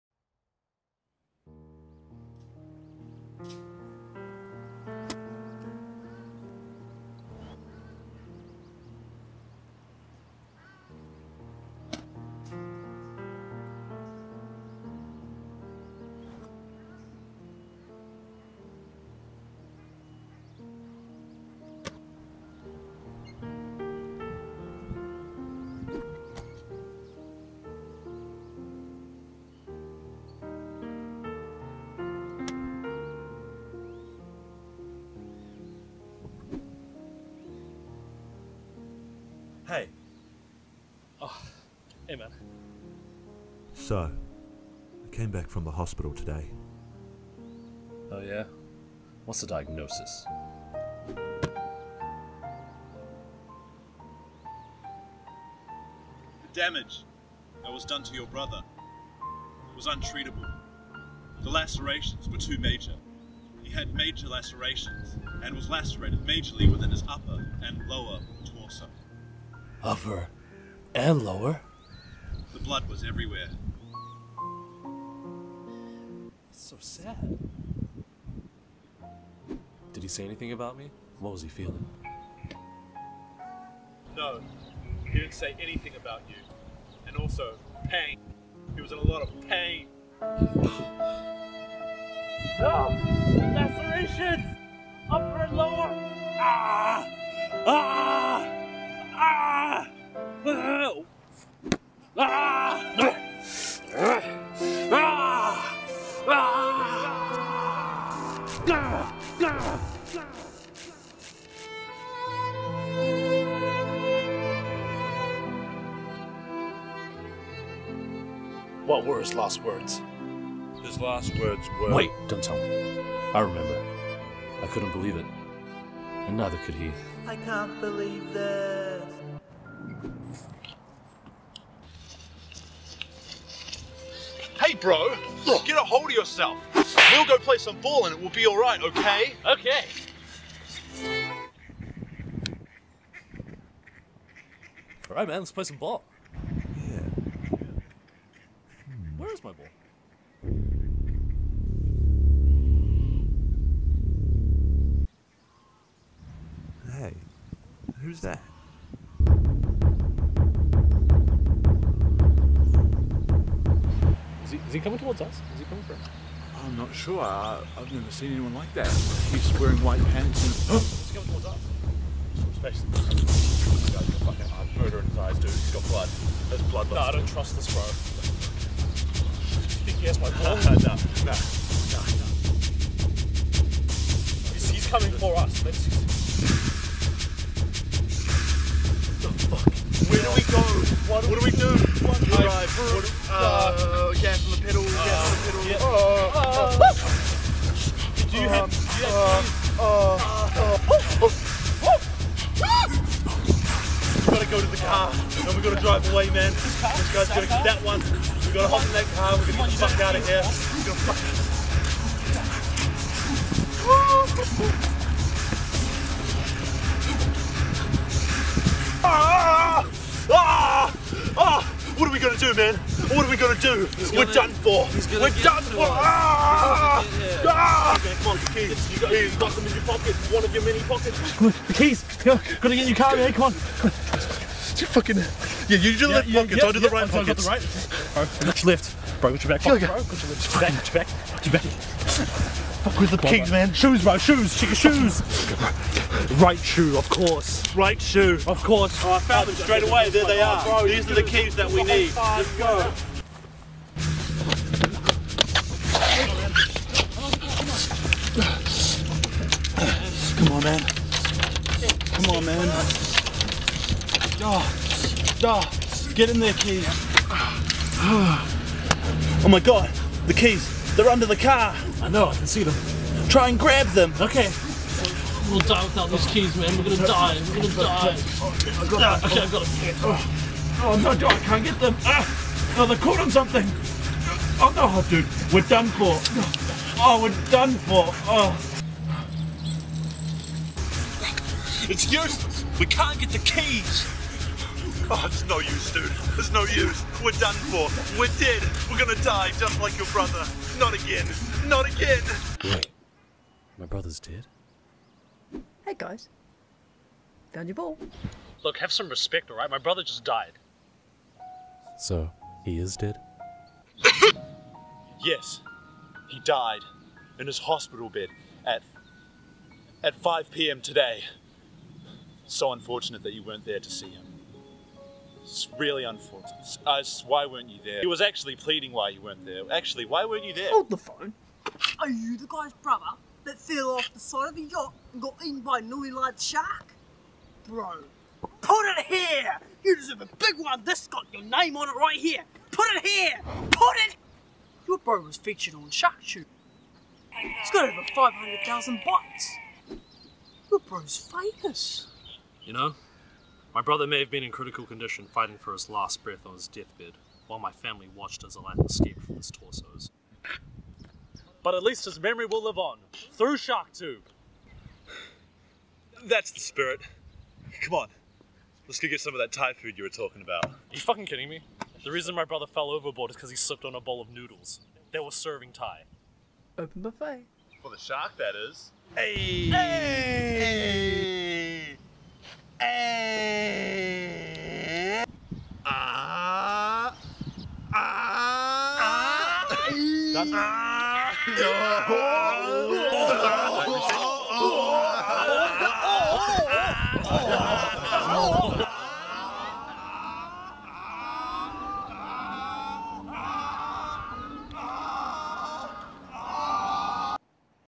[Sketch] Man EATEN by Abnormally large shark!!!! | SharkTube weekly TOP #10's
This WEEK on SharkTube's top #10 Shark attacks,

A man grieves over his brother after he was put in "critical condition" from a FREAK shark attack while partying on their expensive $3.5 Million Yacht they bought from Dubai with the cash their father inherited after his passing back in 2007.  The shark attacked the poor feller after he slipped on a bowl of noodles and fell overboard, to everyone's surprise the shark was BIGGER than normal and swallowed him whole, hospitalizing him with major lacerations. He passed away at 5pm 2 weeks later.

IF YOU LIKED THIS WEEKS ATTACK

- POIP